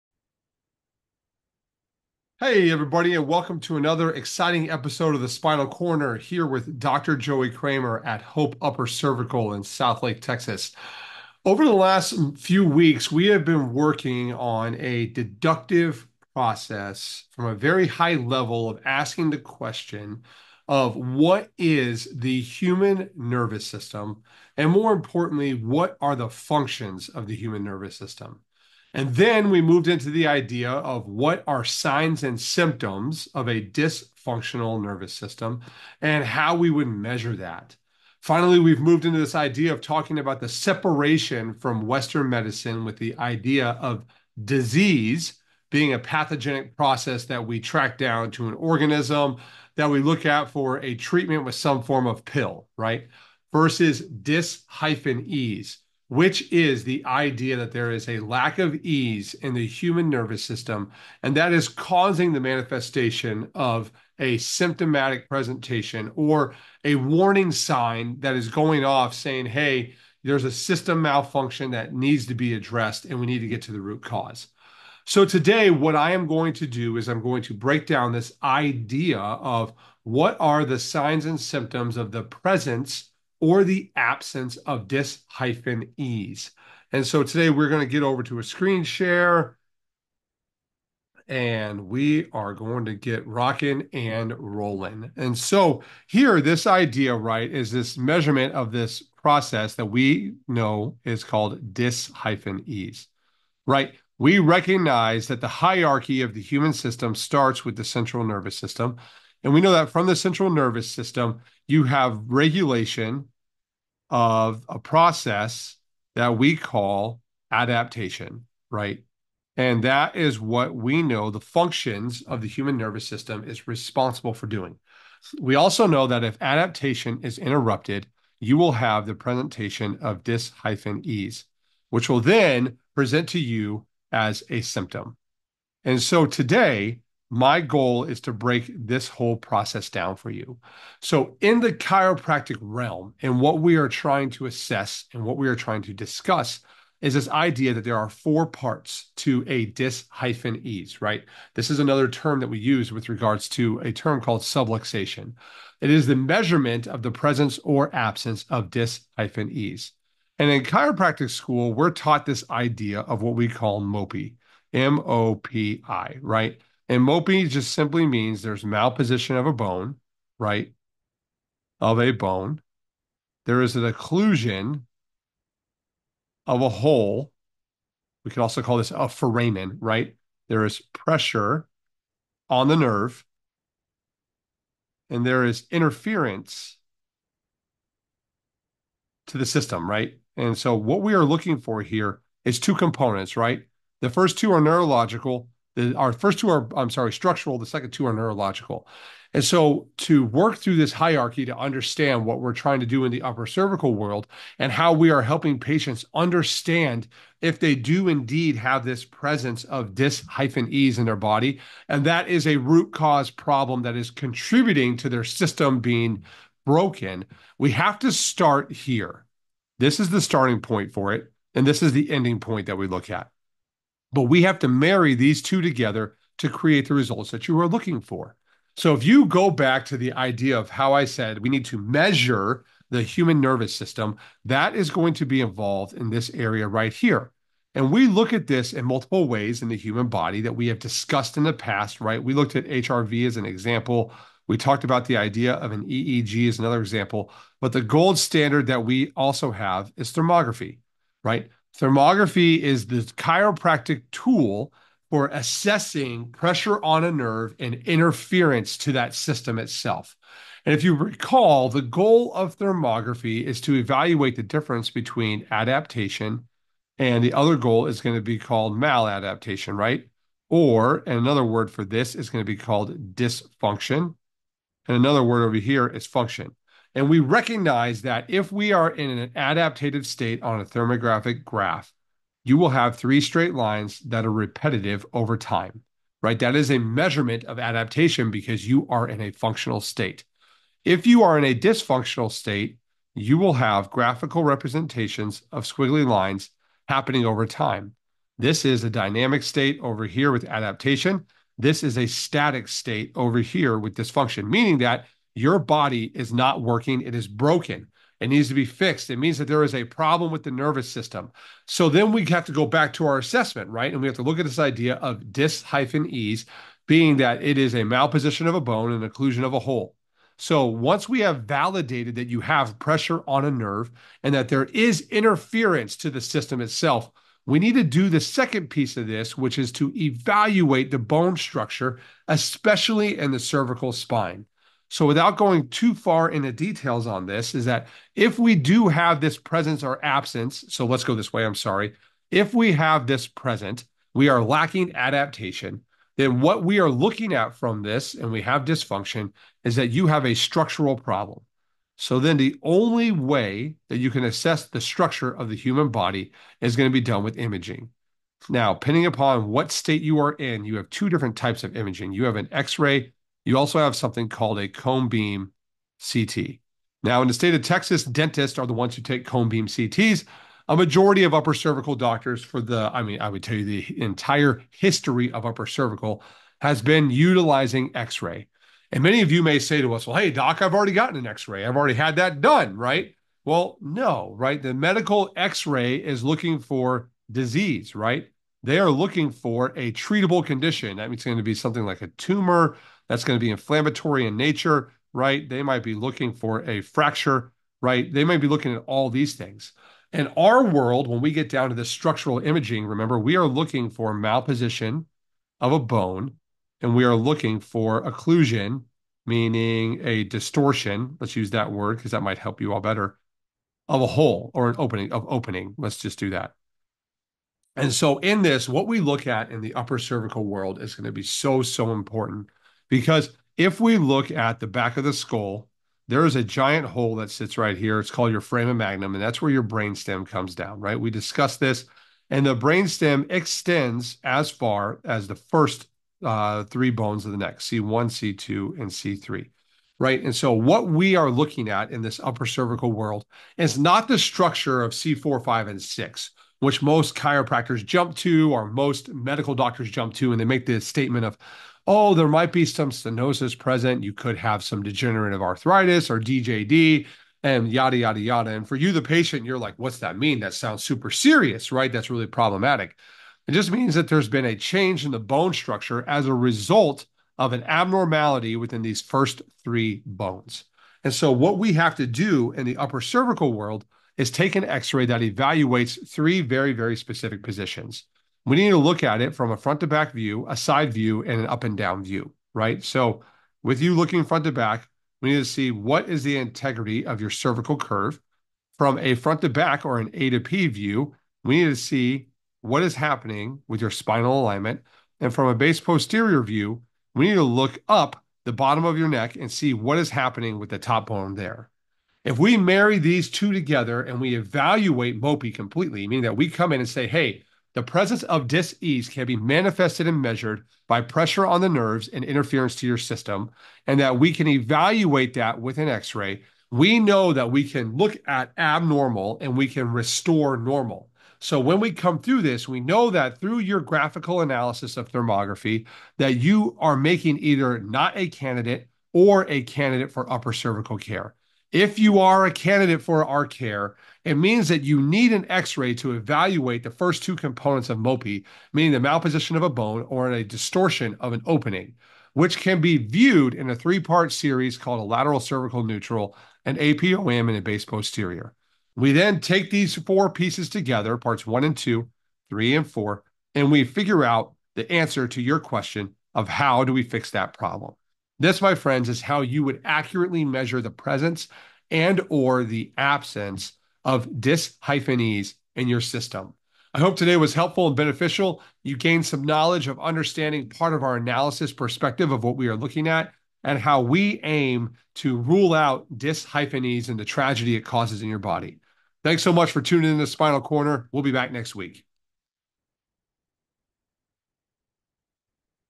The two keys to bio-hacking your nervous system and restoring optimal function.
The internet is full of recommendations for biohacking our health. However, most of these are not a root cause solution. You will find that many people will start a journey embracing these bio hacks and while they may get some relief of symptoms, very few will actually eliminate the issue for the long run. Instead, I propose that we take a different look at the nervous system and address the underlying dysfunction. Then, once this system is fortified with optimal structure, supplement the bio hacks to improve operating capacity.